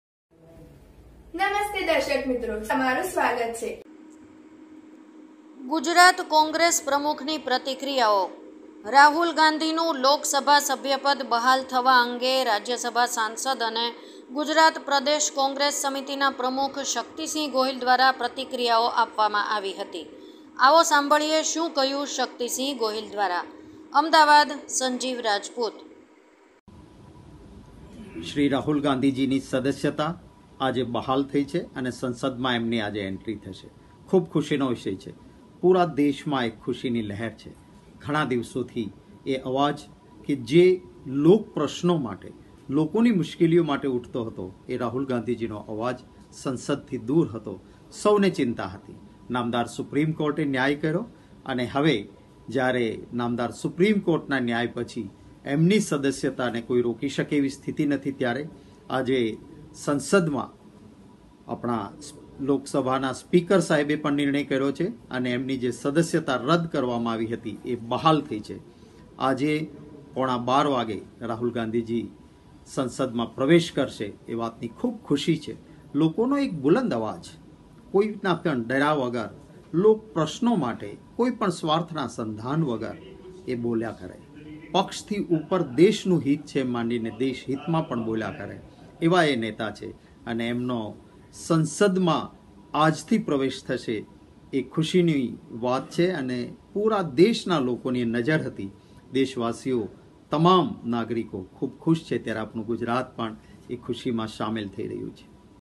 NEWS AHMEDABAD ગુજરાત કોંગ્રેસ પ્રમુખની પ્રતિક્રિયાઓ..
ગુજરાત કોંગ્રેસ પ્રમુખની પ્રતિક્રિયાઓ

રાહુલ ગાંધીનું લોકસભા સભ્યપદ બહાલ થવા અંગે રાજ્યસભા સાંસદ અને ગુજરાત પ્રદેશ કોંગ્રેસ સમિતિના પ્રમુખ શક્તિસિંહ ગોહિલ દ્વારા પ્રતિક્રિયાઓ આપવામાં આવી હતી આવો સાંભળીએ શું કહ્યું શક્તિસિંહ ગોહિલ દ્વારા..

અમદાવાદ
સંજીવ રાજપૂત